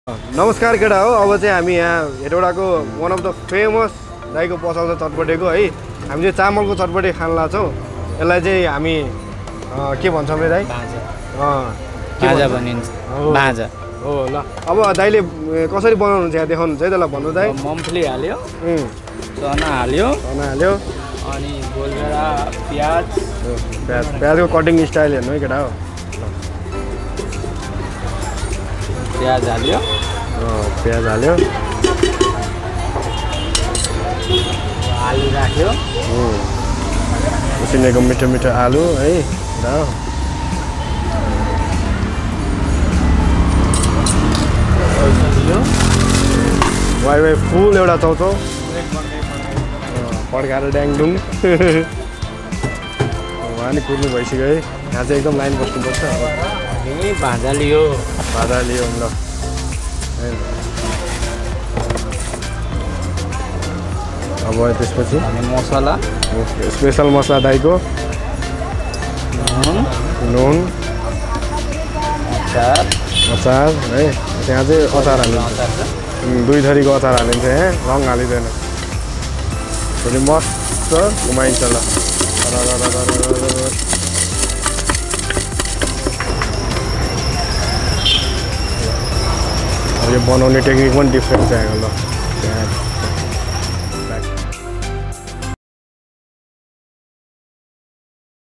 Vlog S1E6: Chatpatey Unleashed - A Hidden Street Food Gem!
Embark on a culinary journey as we dive deep into the bustling streets to uncover a hidden gem - the iconic "Chatpatey". In this episode, we embrace the vibrant Indian food vlogging style to truly capture the essence and flavors of this underrated masterpiece.

Chatpatey is not just a dish; it's an emotion, a burst of flavors, a celebration of street food craftsmanship. For those who haven't yet had the joy of experiencing this treat, this episode is a heartfelt invitation to explore and relish this Nepalese delight.

A big shoutout to the incredible street food vendor who passionately brings this dish to life every single day. Let's rally behind our local heroes and put Chatpatey on the culinary map where it truly belongs!

Join us in Vlog S1E6 as we champion local flavors and sing praises for the unsung heroes of the street food world!